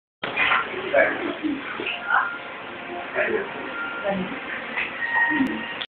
【ラーメンWalker】麻麻（岐阜県）
醤油ラーメン、チャーシューがとろけます。あっさりだけど、しっかりしたスープは、おいしくて飲み干してました！どて丼も絶品です。餃子はニンニクたっぷりでスタミナつきますよ〜＼(^O^)／